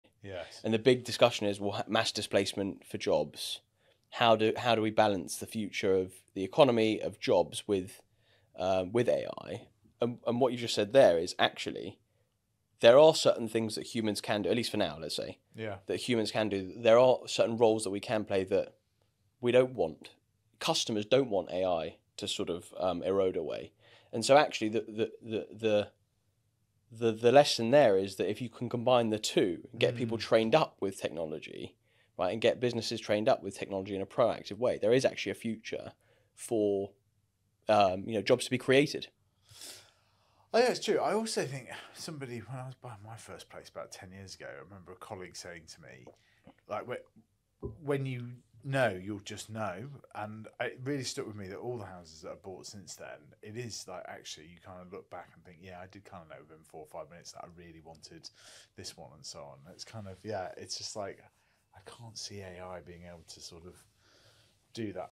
Balancing the Future of Work- Humans, AI, and the Intuitive Decisions
Artificial intelligence is undeniably transforming our world, both in how we live and work. This shift is already underway, yet there remain numerous fields where AI won't take over, and countless new opportunities are emerging. Mastering the art of integrating AI into your professional life is crucial for harnessing these possibilities.

AI advancement is reshaping our daily lives and professional landscapes, offering a wealth of new prospects while leaving certain areas untouched. Embracing this technology and learning to incorporate it into your work effectively is key to unlocking its full potential.